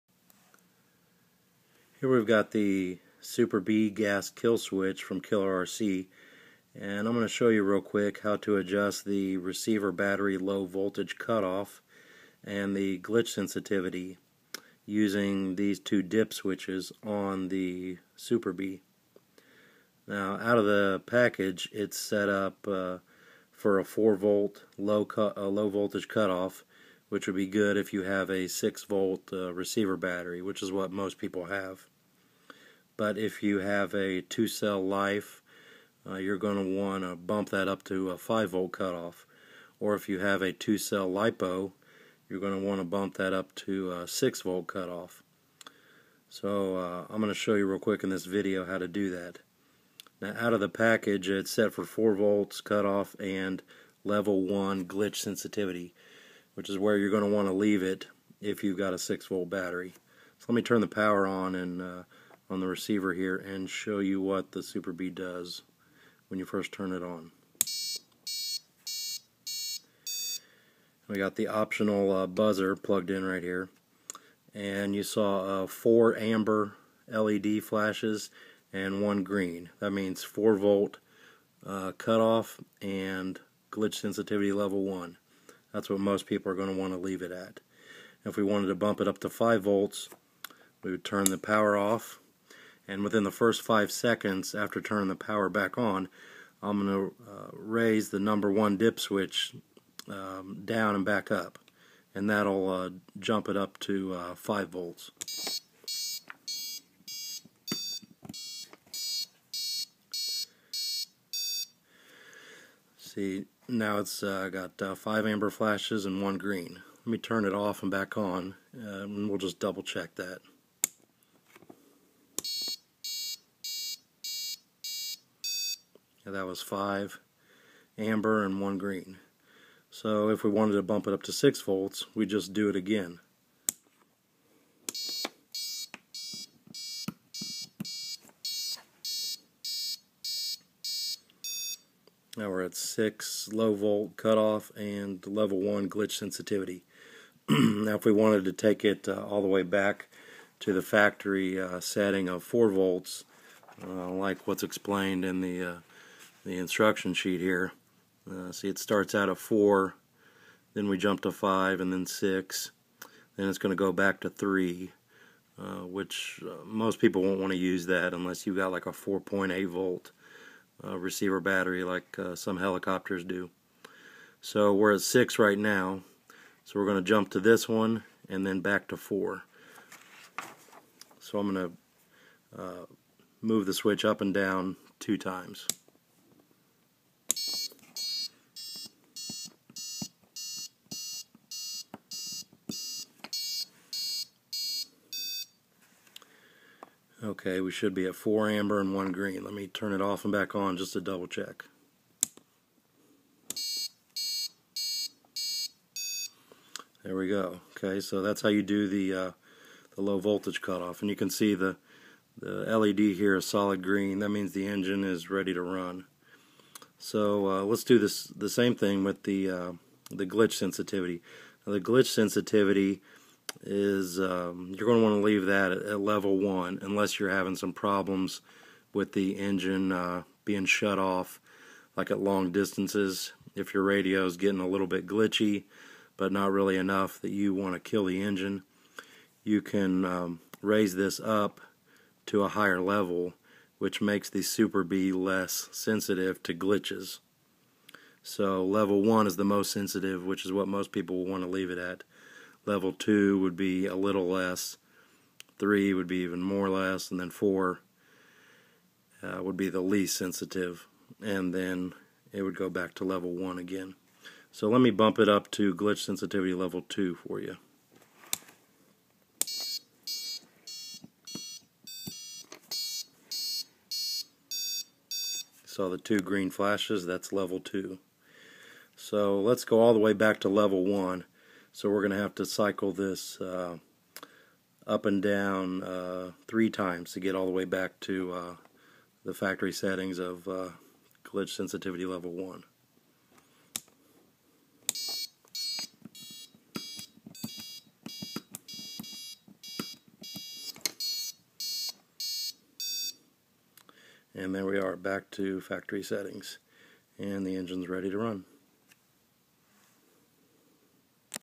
Super Bee low voltage cutoff & glitch sensitivity, Gas RC Kill Switch, Killer RC
I explain how to adjust the low voltage cutoff and glitch sensitivity on the Super Bee gas kill switch. The Super Bee has 3, 4, 5, or 6 volt low voltage cutoff for the receiver battery on a gas powered large-scale RC car. Setting the voltage cutoff will protect your battery from being over-discharged. for more info or to purchase a Super Bee.